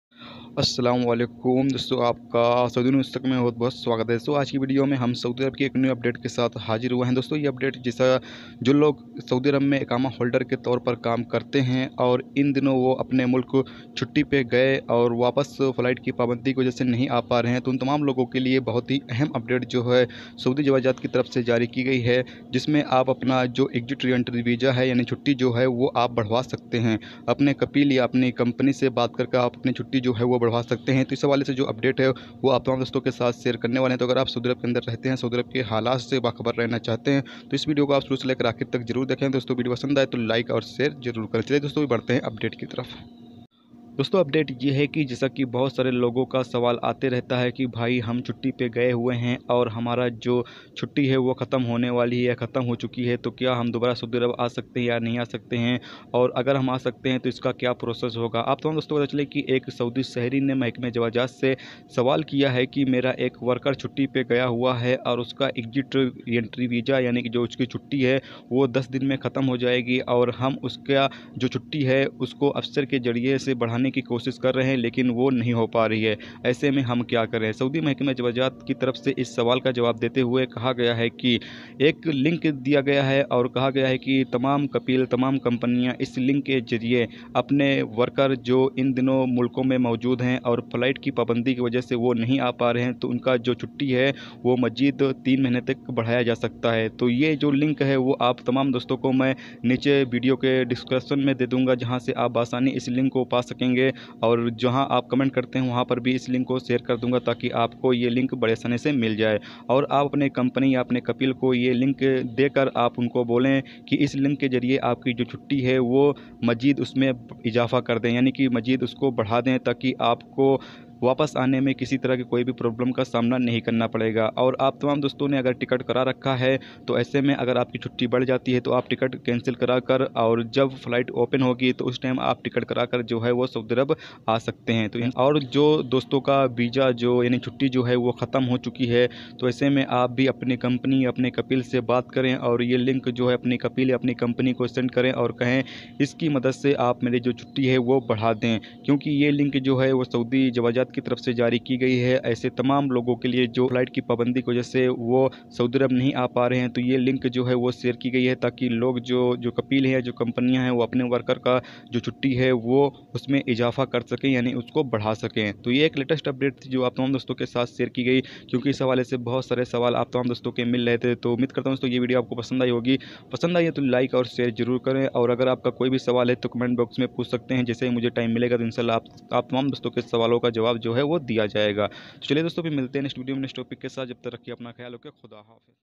Saudi Arabia Big Letest News Update For Exit Re Enter Visa Extension (31-3-20) Saudi Today Newspaper
Assalamalaikum Dosto,,

Saudi Arabia Daily News Hindi Urdu,,
Saudi Arabia Today News Hindi Urdu,,